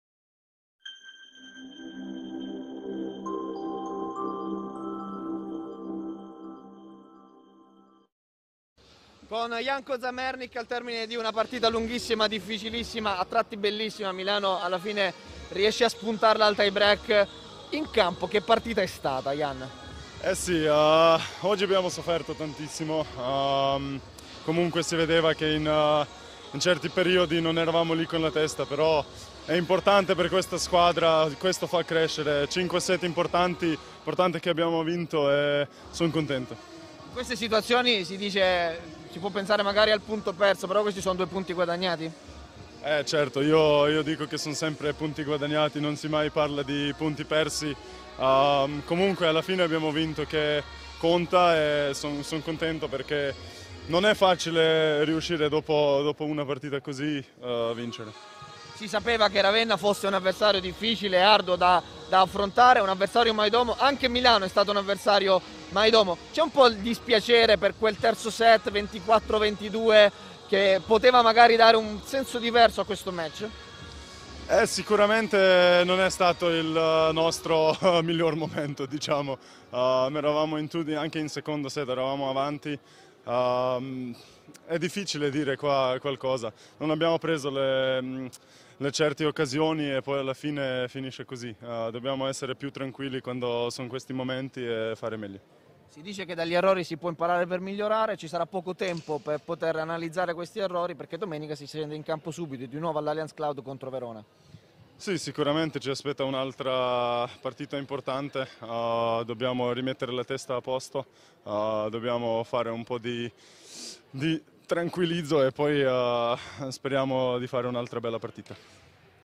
Superlega, l'intervista post Milano - Ravenna di Jan Kozamernik
🎙 | INTERVISTA
Le parole a fine gara di Jan Kozamernik dopo la vittoria per 3-2 dell'Allianz Powervolley Milano contro Ravenna.